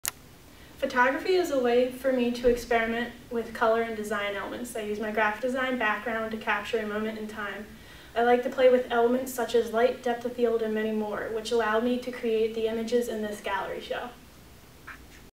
Lindsey Piller Artist Statement
Lindsey Piller discusses her gallery show